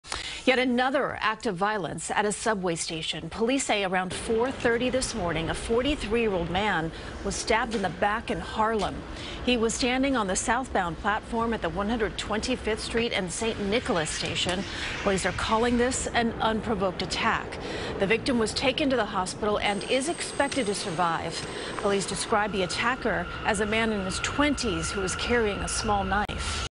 Man stabbed in back while waiting for subway in Harlem
Police say a man was stabbed in the back at a Harlem subway station early Friday morning.